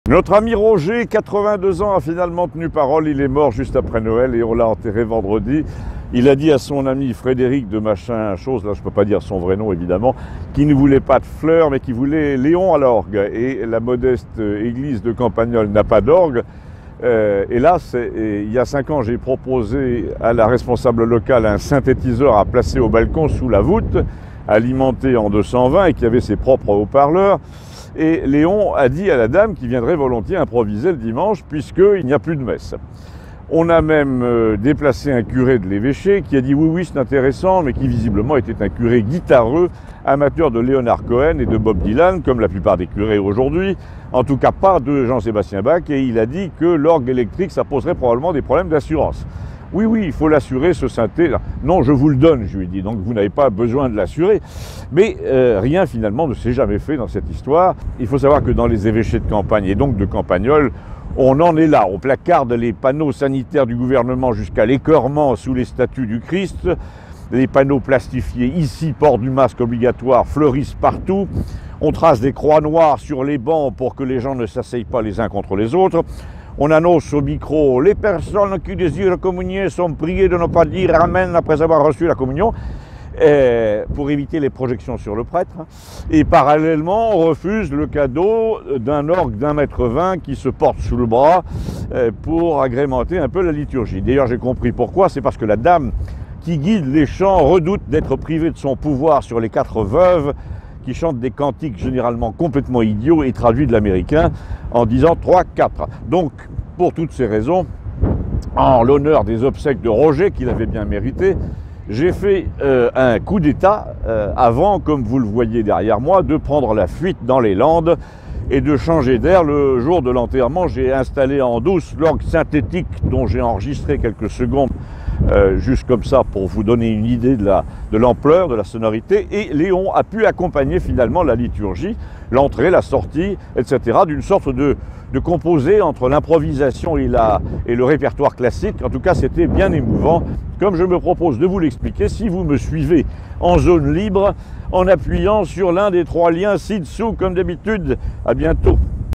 La vague scélérate est en route mais ils font des selfies sur la plage
pour avoir accès aux contenus interdits aux Français